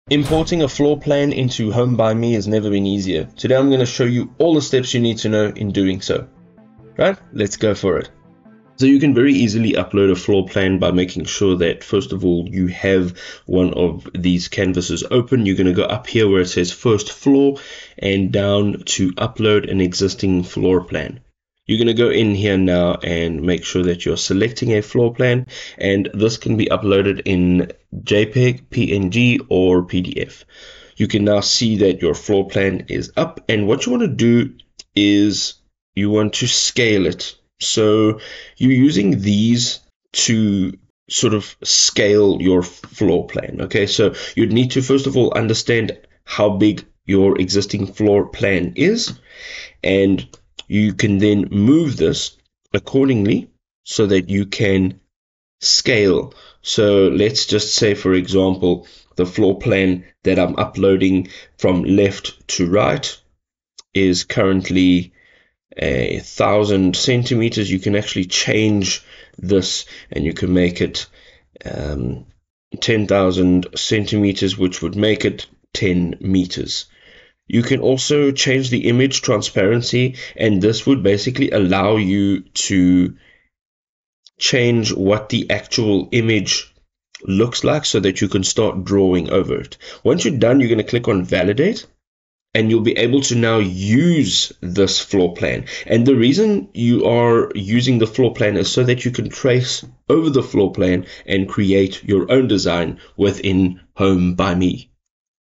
How to Import a Floor Plan in HomeByMe | HomeByMe Tutorial 2025
In this video, "How to Import a Floor Plan in HomeByMe," we’ll guide you through the process of bringing your architectural designs to life by importing existing floor plans into the platform. This feature allows you to start your projects with a solid foundation, making it easier to visualize and customize your space. We’ll walk you through the steps to upload your floor plan, adjust its dimensions, and trace over it to create a 3D model. Whether you’re working on a renovation or designing a new build, this tutorial will help you efficiently transition from 2D to 3D.

Timestamps

Intro – 00:00 – 00:08
Importing a Floor Plan in HomeByMe – 00:09 – 01:45